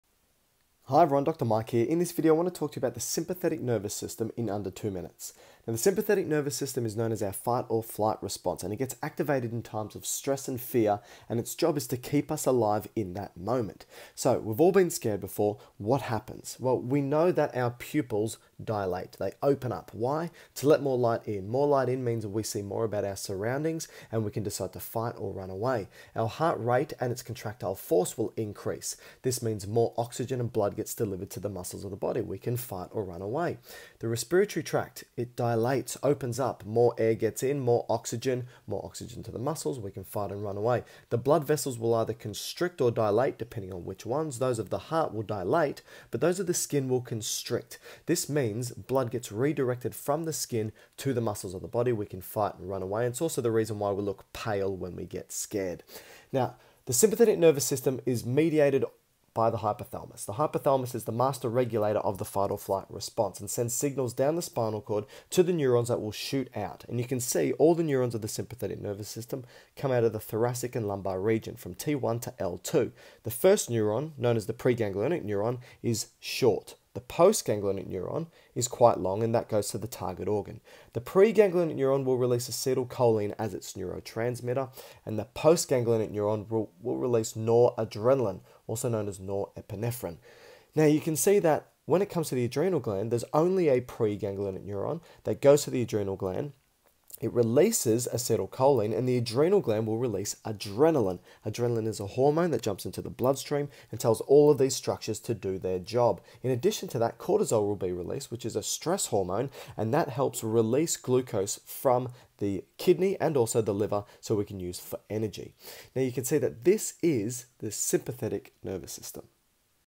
Sympathetic Nervous System in under 2 mins!
In this video, Dr Mike discusses how the sympathetic nervous system works to keep you alive, in under 2 minutes!